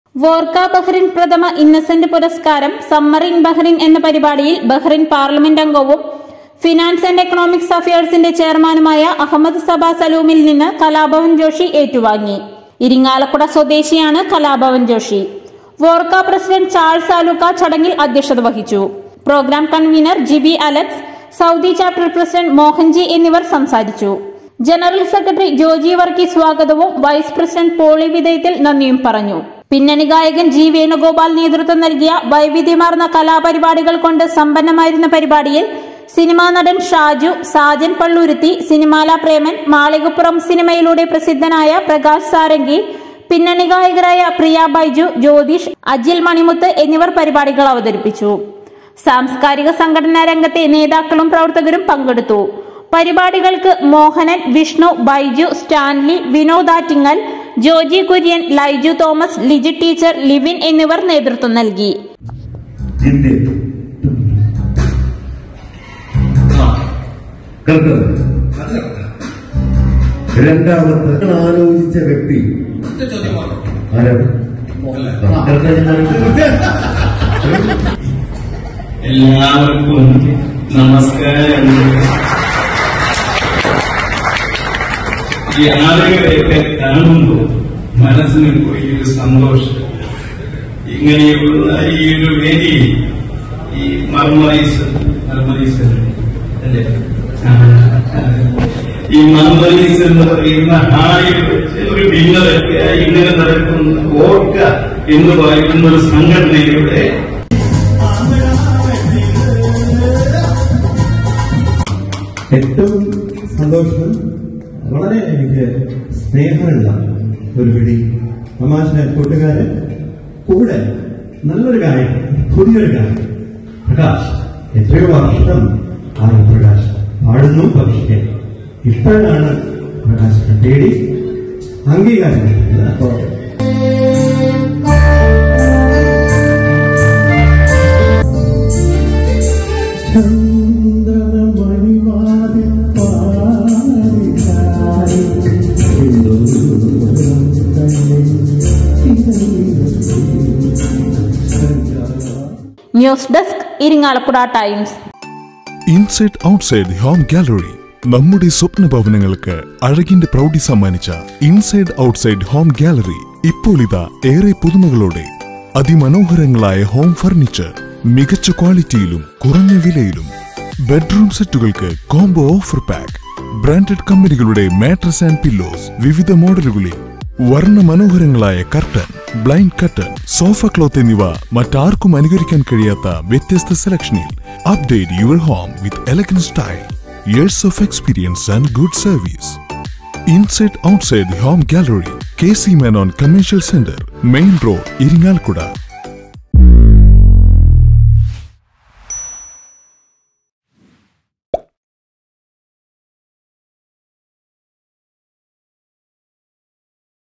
വോർക്ക ബഹറിൻ പ്രഥമ ഇന്നസെന്റ് അവാർഡ് കലാഭവൻ ജോഷിക്ക് സമ്മാനിച്ചു...
ഇരിങ്ങാലക്കുട : വോർക്ക ബഹറിൻ പ്രഥമ ഇന്നസെന്റ് പുരസ്കാരം "സമ്മർ ഇൻ ബഹറിൻ" എന്ന പരിപാടിയിൽ ബഹറിൻ പാർലമെൻറ് അംഗവും ഫിനാൻസ് & എക്കണോമിക്സ് അഫേഴ്സിന്റെ ചെയർമാനുമായ അഹമ്മദ് സബാ സലൂമിൽ നിന്നും കലാഭവൻ ജോഷി ഏറ്റുവാങ്ങി.

ഇരിങ്ങാലക്കുട സ്വദേശിയാണ് കലാഭവൻ ജോഷി.